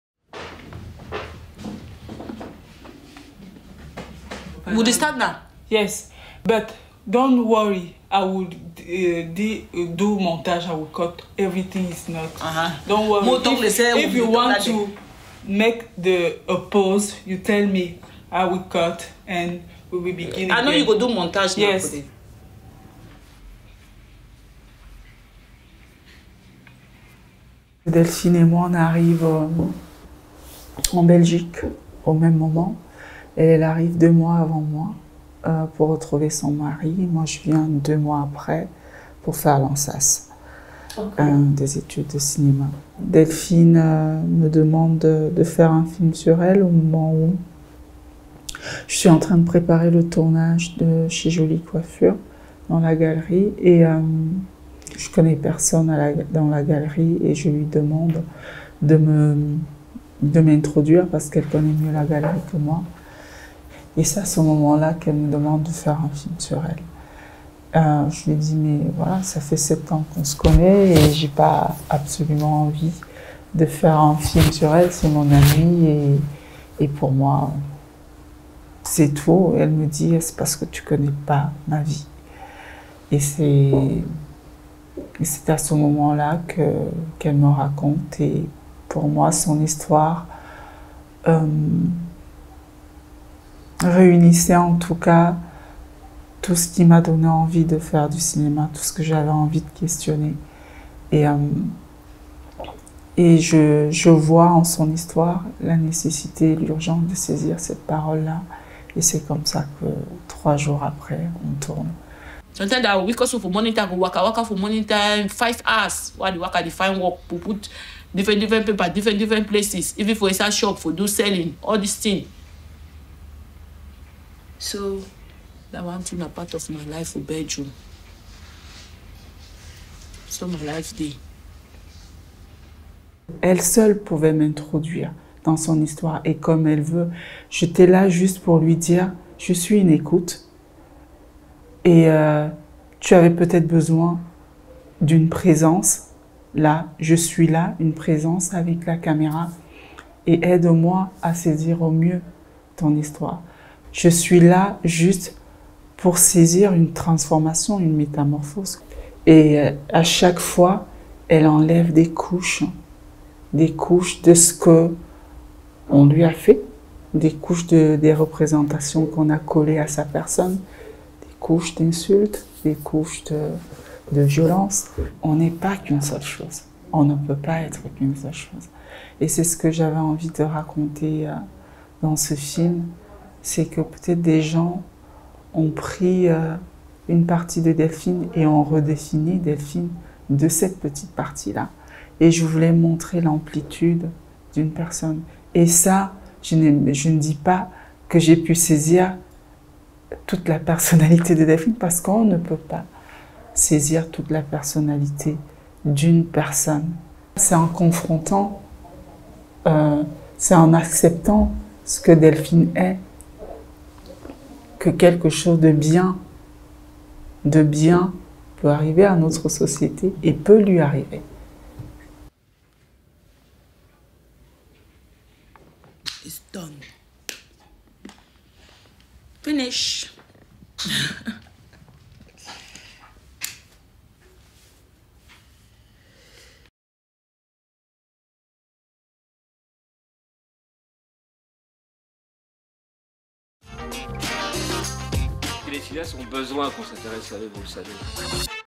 Rosine Mbakam à propos des Prières de Delphine
De son amie dont elle recueille l’histoire, les colères, les douleurs, elle fait un portrait bouleversant, celui d’une femme qui retrouve sa dignité en racontant toutes les violences qui lui ont été faites. Calme et posée, avec une certaine douceur et beaucoup de détermination, Rosine Mbakam met en jeu dans chacun de ses films son travail de réalisatrice qui consiste, lui aussi, à s’affranchir des rôles assignées par une société traversée de profonds schémas de domination.